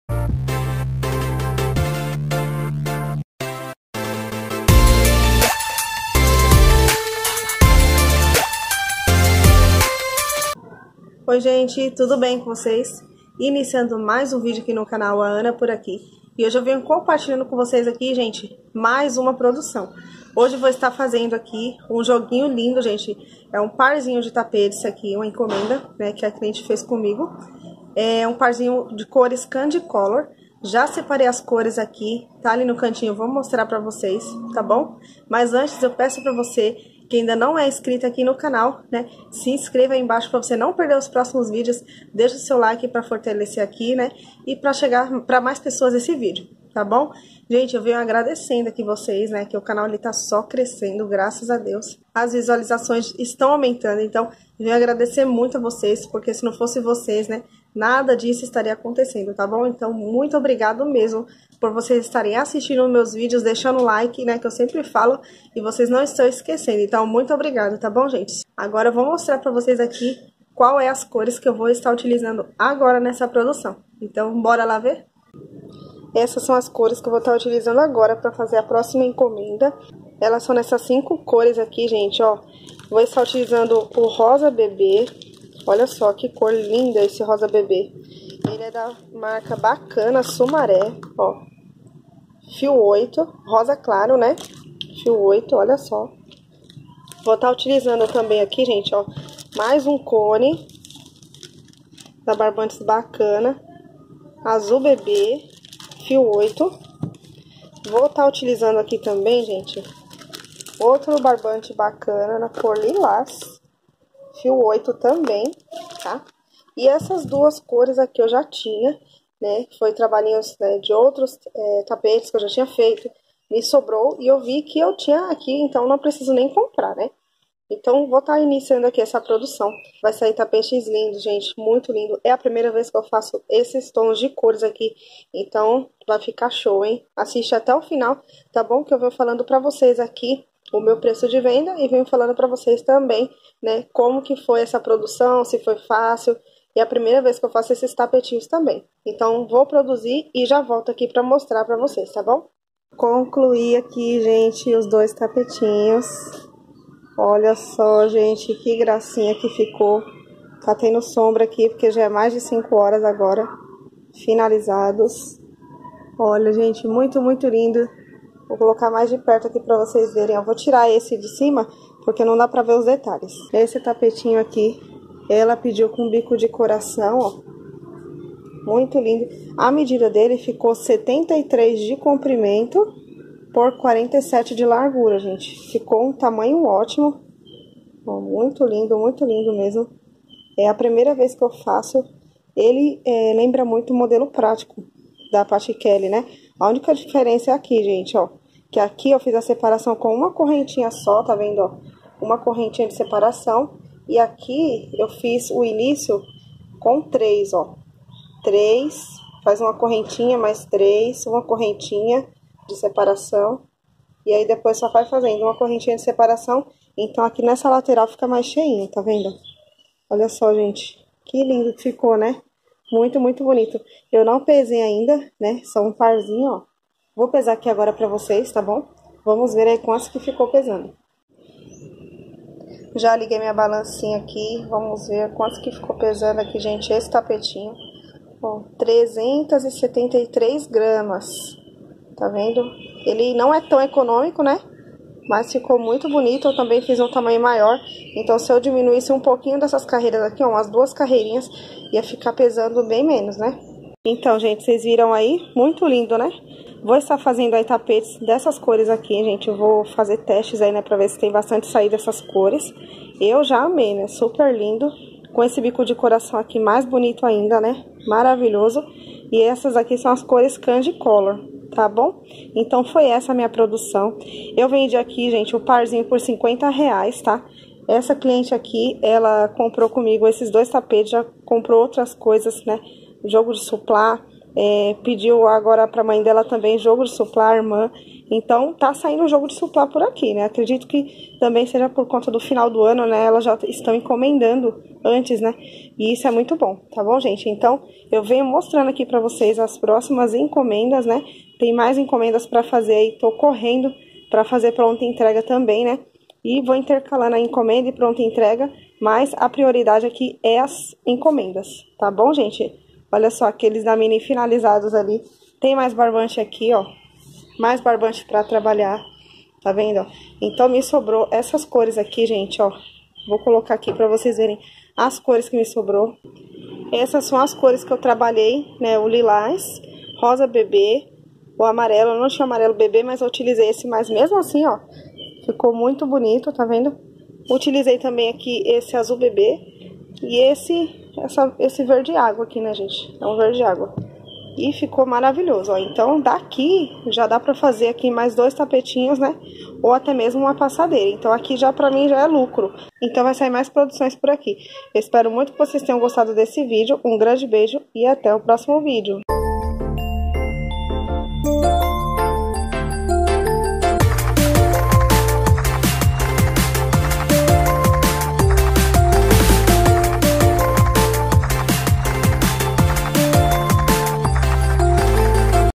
😍ENCOMENDA NAS CORES CANDY COLOR OLHA QUE LINDO 👏
Neste vídeo compartilhei com vocês uma produção maravilhosa nas cores candycolor, fiz um parzinho de tapetes com bico de coração ❤️ pensa em uma encomenda linda que ficou assiste até o final.

💟 Segue lá no Instagram compartilho meu dia a dia no ateliê.